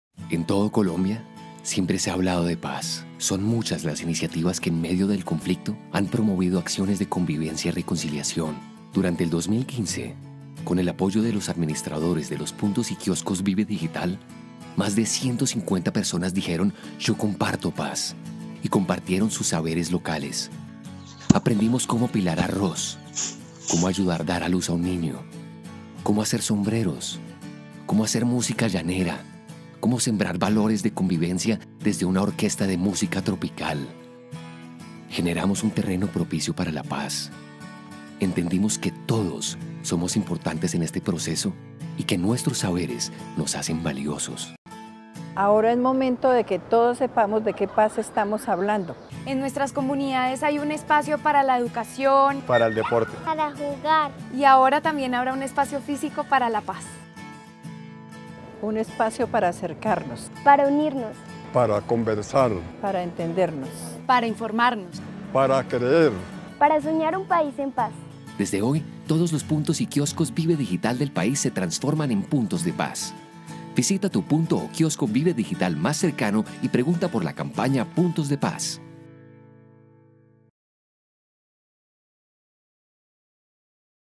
1.123 Puntos y Kioscos Vive Digital ahora serán
Con un total de 1.123 Puntos y Kioscos Vive Digital de 29 departamentos del país, el Ministerio TIC pone a disposición estos escenarios para que los colombianos puedan participar en jornadas de conversación alrededor del proceso de paz.